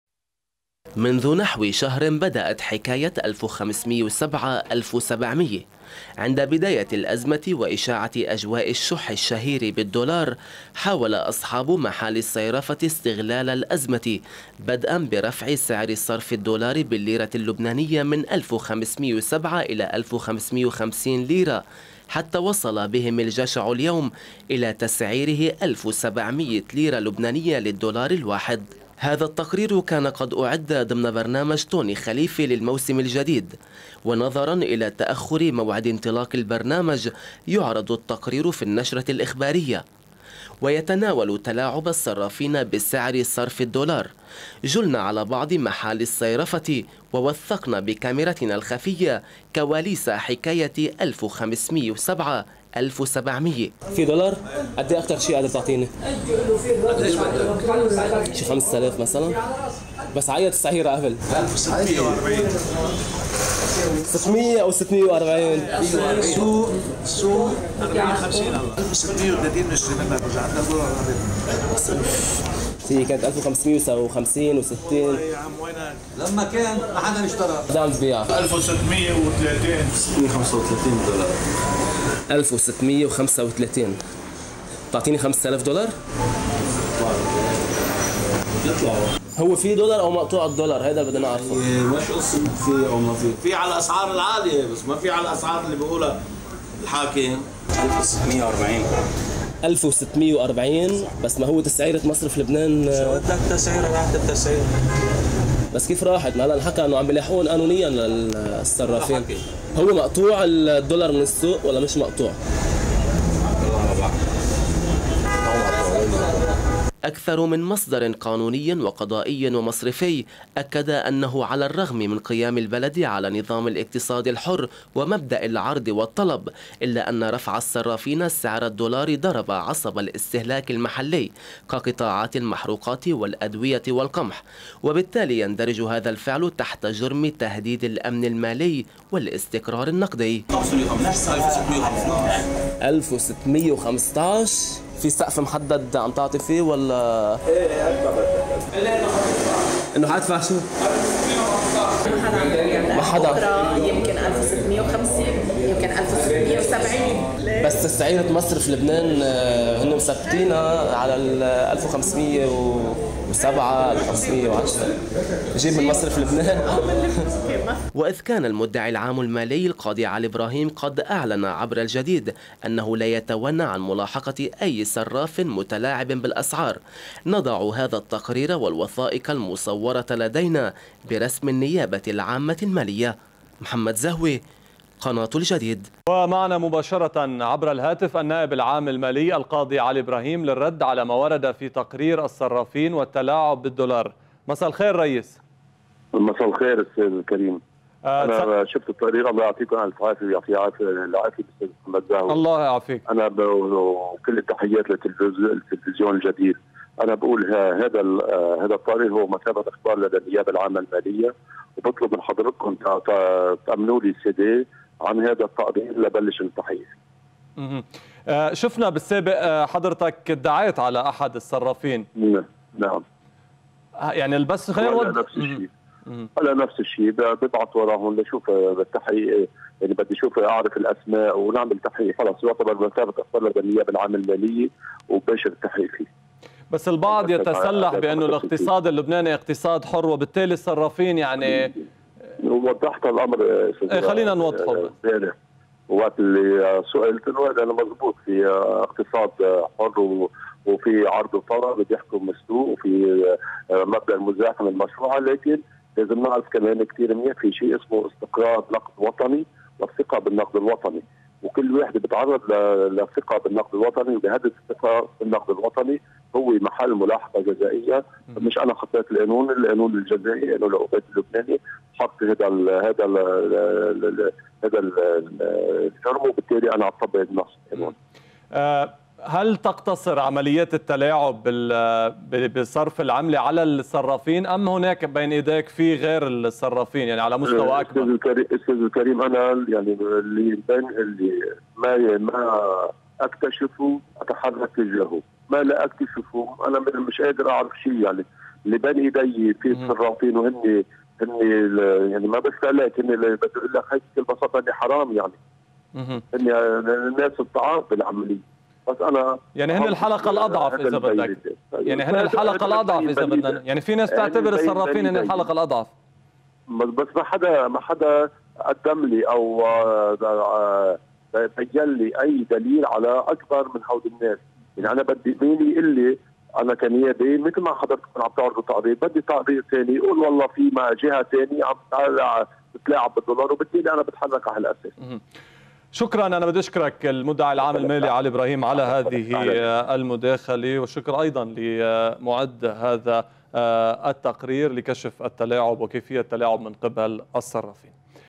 الجديد تدخل مغارة الصيرفة .. وتوثق تلاعب الصرافيين بأسعار الدولار! – محمد زهوة
منذ نحو شهر بدأت حكاية الألف والخمسمئة وسبعة - الالف والسبعمئة.
عند بداية الأزمة وإشاعة أجواء الشح الشهير بالدولار، حاول أصحاب محال الصيرفة استغلال الأزمة بدءا برفع سعر صرف الدولار بالليرة اللبنانية من الف وخمسمئة وسبعة الى الف وخمسمئة وخمسين ليرة حتى وصل بهم الجشع اليوم الى تسعيره بألف وسبعمئة ليرة لبنانية للدولار الواحد.
هذا التقرير كان قد أعد للعرض ضمن برنامج طوني خليفة للموسم الجديد/ ونظرا الى تأخر موعد انطلاق البرنامج يعرض التقرير في النشرة الإخبارية ويتناول تلاعب الصرافين بسعر صرف الدولار, جلنا على بعض محال الصيرفة ووثقنا بكاميرتنا الخفية كواليس حكاية الف وخمسمئة وسبعة الف وسبعمئة.
- الفيديوهات المصورة بالكاميرا الخفية
أكثر من مصدر قانوني وقضائي ومصرفي، أكد أنه على الرغم من قيام البلد على نظام الاقتصاد الحر ومبدأ العرض والطلب، إلا أن رفع الصرافين سعر الدولار ضرب عصب الاستهلاك المحلي كقطاعات المحروقات والأدوية والقمح، وبالتالي يندرج هذا الفعل تحت جرم تهديد الامن المالي والاستقرار النقدي.
وإذ كان المدعي العام المالي القاضي علي ابراهيم قد أعلن عبر الجديد أنه لا يتوانى عن ملاحقة أي صراف متلاعب بالأسعار، نضع هذا التقرير والوثائق المصورة لدينا برسم النيابة العامة المالية.